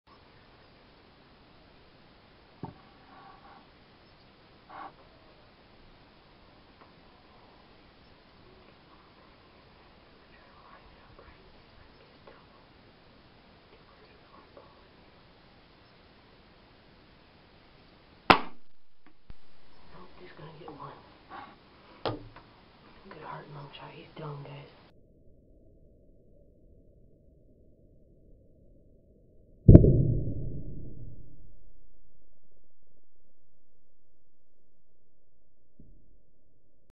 sparrow kill slow motion
(WARNING GRAPHIC!!)house sparrow kill with the gamo DX i was trying to get a double kill but the bird in the back moved at the very last second, i found out how to make awesome slow mo videos so we will have more of these coming soon.. we are now working on scopecam videos,if it flys it dies.. VIEWER DISCRETION ADVISED